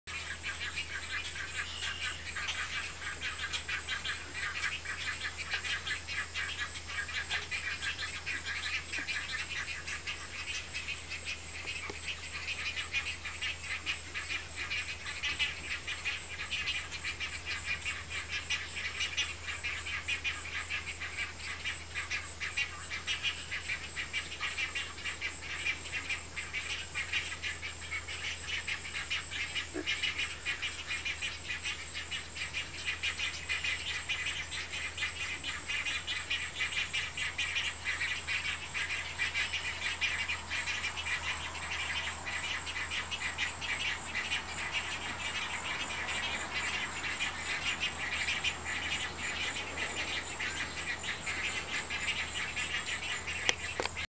Great Egret feeding chicks
Great Egret mother feeding large chicks in a nest.